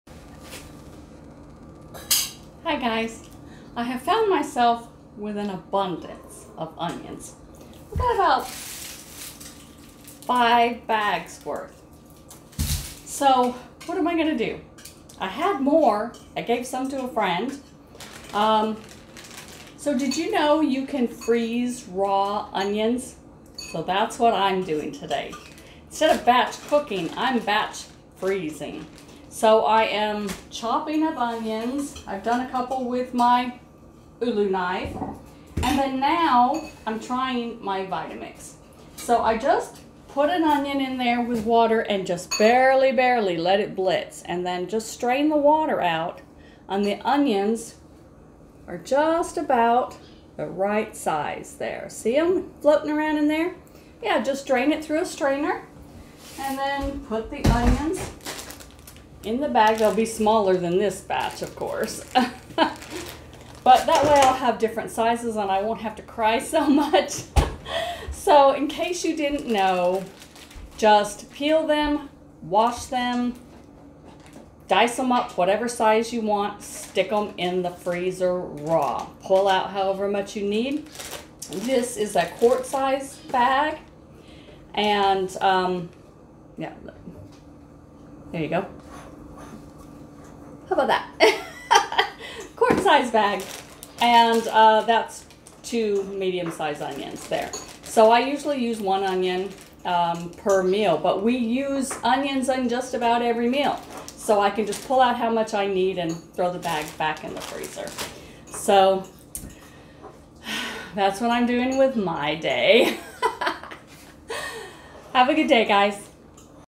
Freezing Onions
What to do when you find a massive sale on onions? FREEZE them! Here is how.